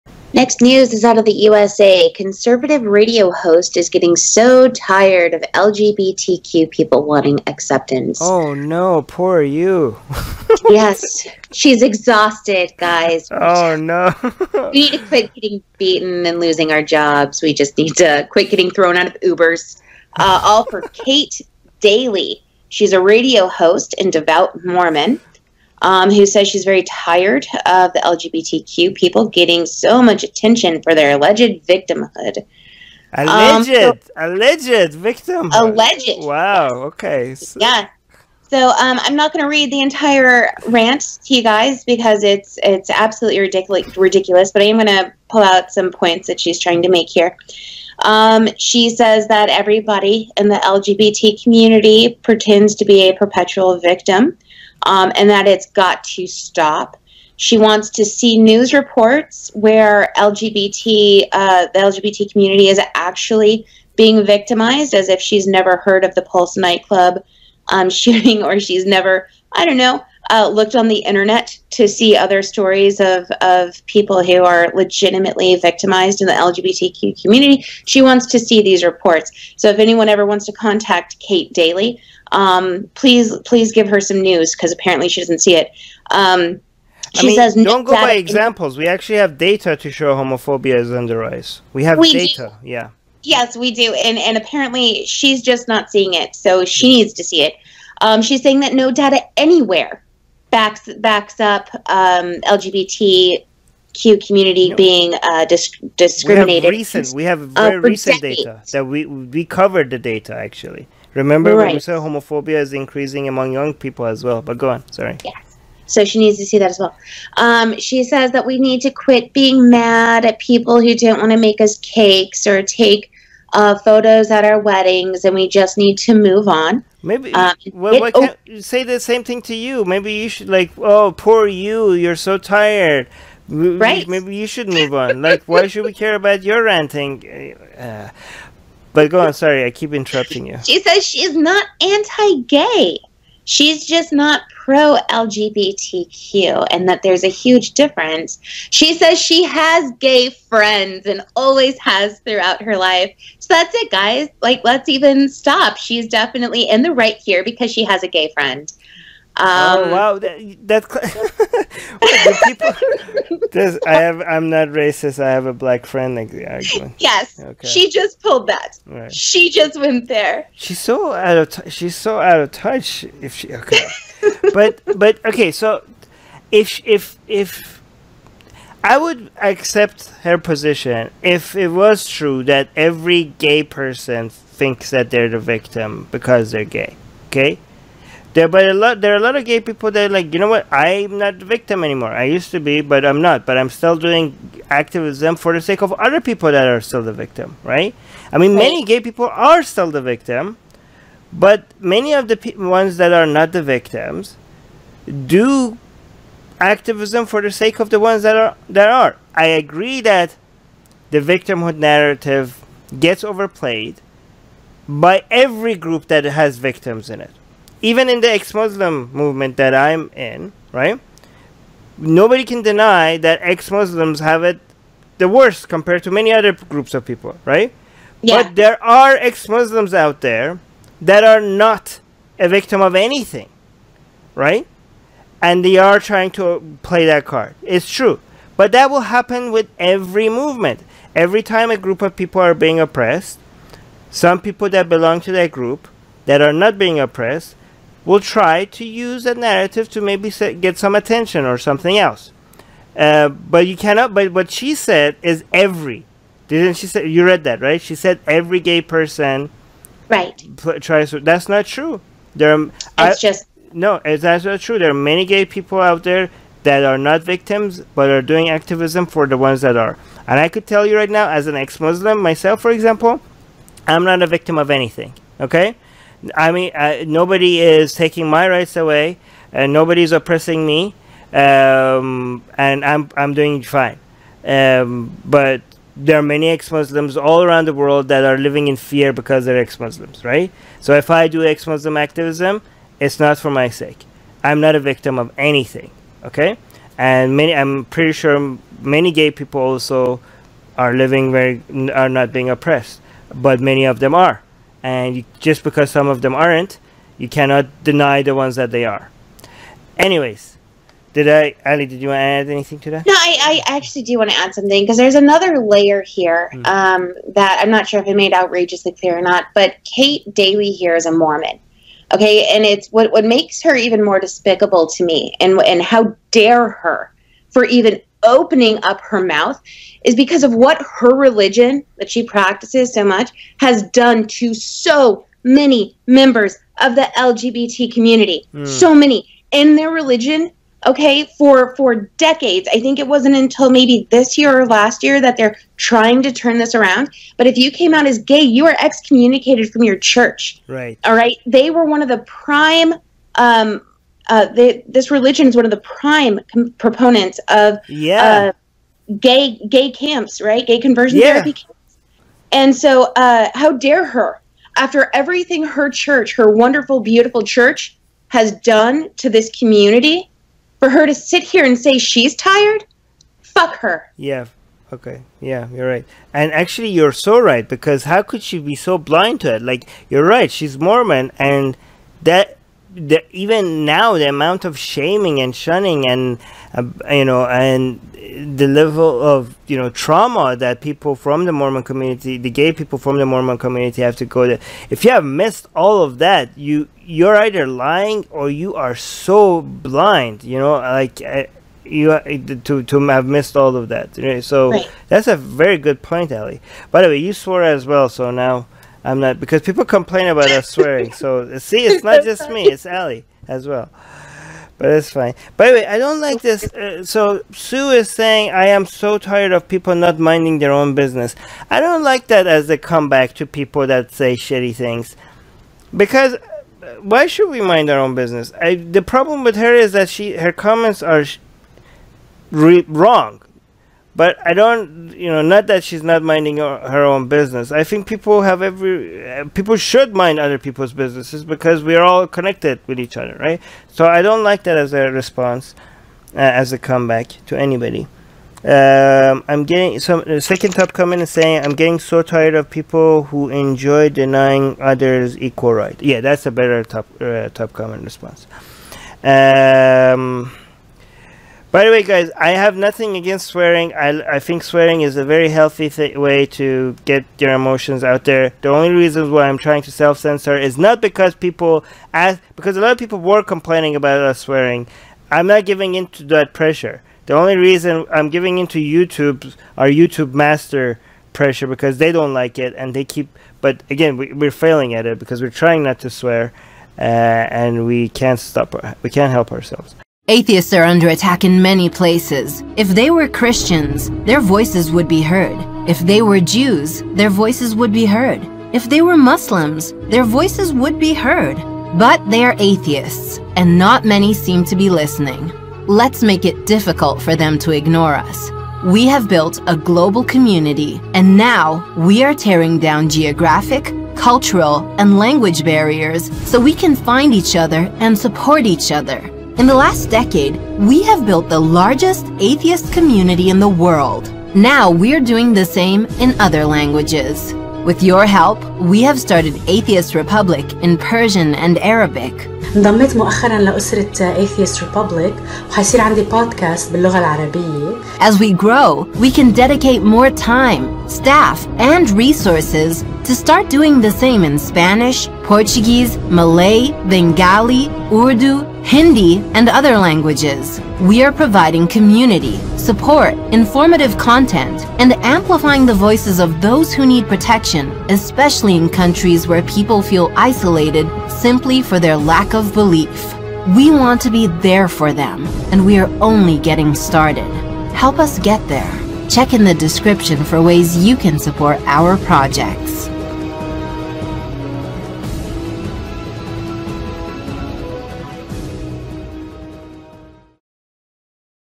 Conservative Radio Host is “Getting So Tired” of LGBTQ People Wanting Acceptance 🌈
Kate Hefley Dalley, a radio host and devout Mormon, said that she's very tired of LGBTQ people getting soooooo much attention for their alleged victimhood. A part of her Facebook post says, "Dear LGBTQ Community: Can we all just be honest? I’m getting so tired of this. The perpetual victim thing has got to stop. Your claims of getting fired from every job, beaten up every time you leave the house, everyone hating on you and being thrown out of every home rental — please show me the news reports where this is ACTUALLY happening to you. No data anywhere backs this up for decades. Individual cases here and there does not mean it’s happening to you personally on a daily basis."

Source: Conservative Radio Host is “Getting So Tired” of LGBTQ People Wanting Acceptance
Location: USA